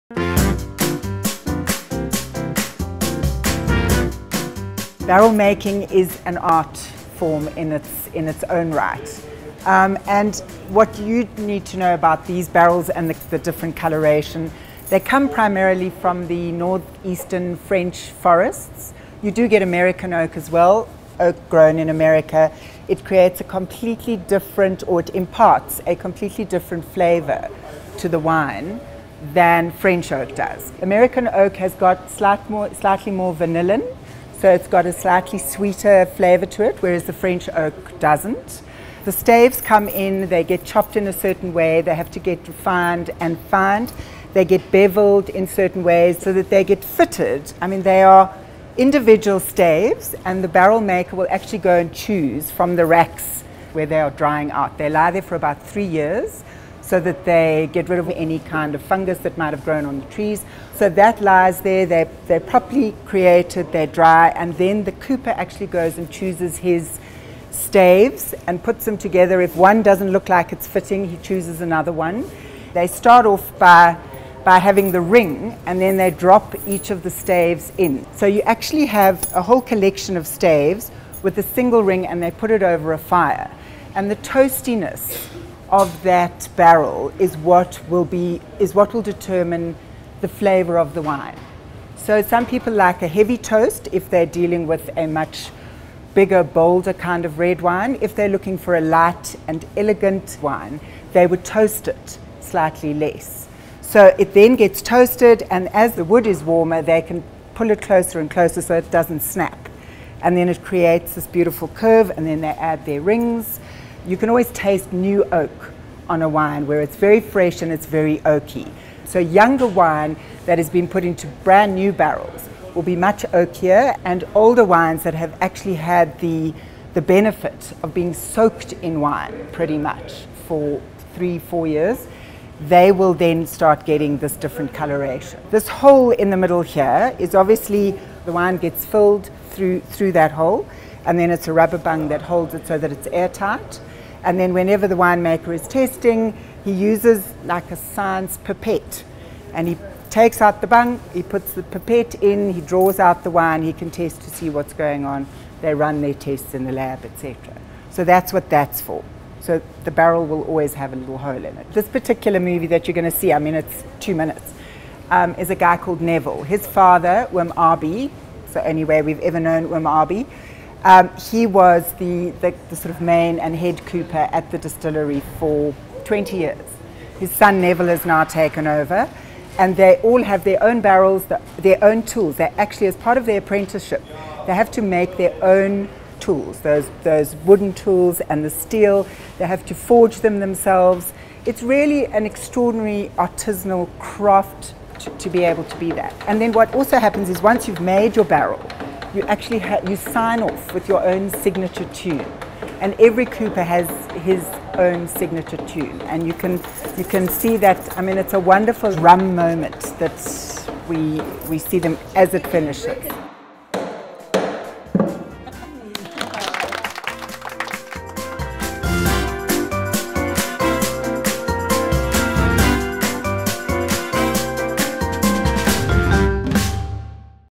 The Art Of Barrel Making - RMB WineX
The perfect barrel forms the foundation of any great wine. From the staves and hand-made tools, to the toastiness infused when the wood is bent over a fire, it is an extraordinary, artisanal display of craftmanship. This year RMBWineX in collaboration with South Hill Barrel Art initiative, presented by RMB Private Bank celebrates every factor involved in the art of winemaking from inception to when it hits the palates of Joburg’s discerning wine lovers.